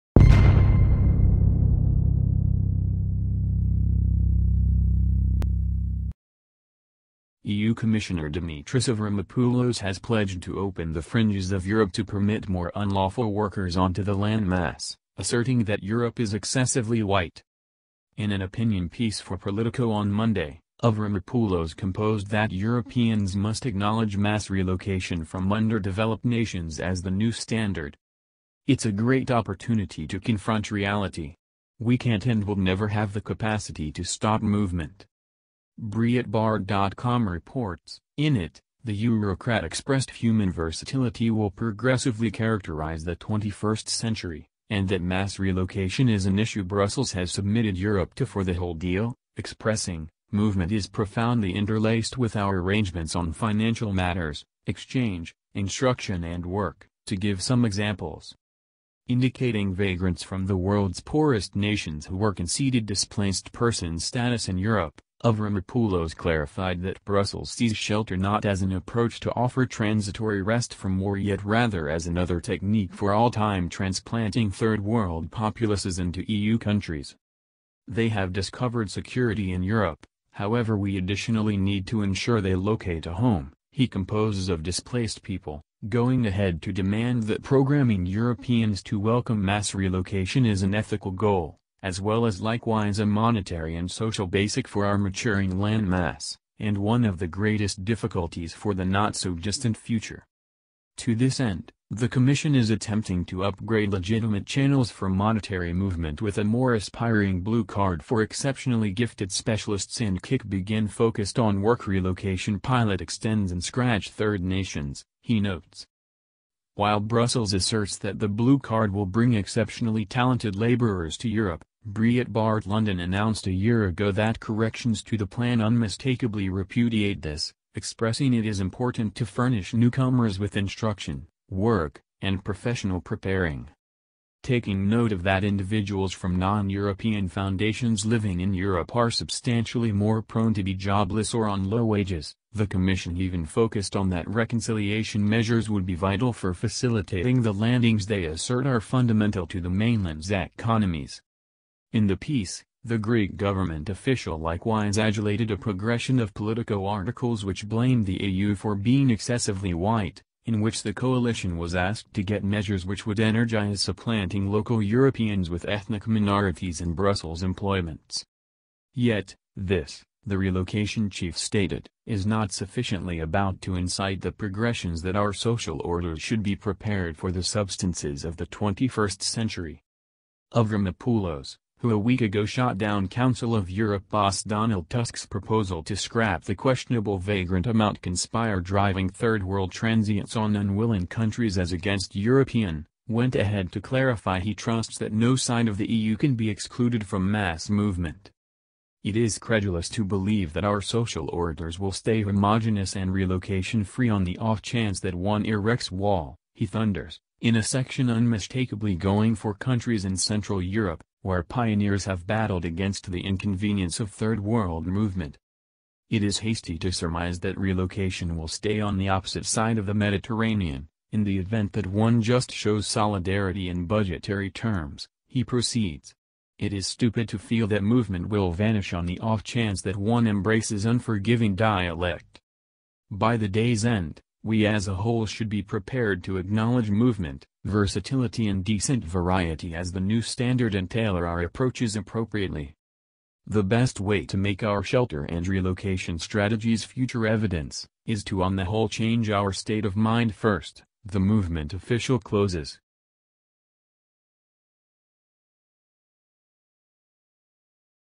EU Leader - ‘Europe Is Too White, More Illegal Immigrants Needed’
EU Commissioner Dimitris Avramopoulos has vowed to open the borders of Europe to allow more illegal immigrants onto the continent, claiming that ‘Europe is too white.’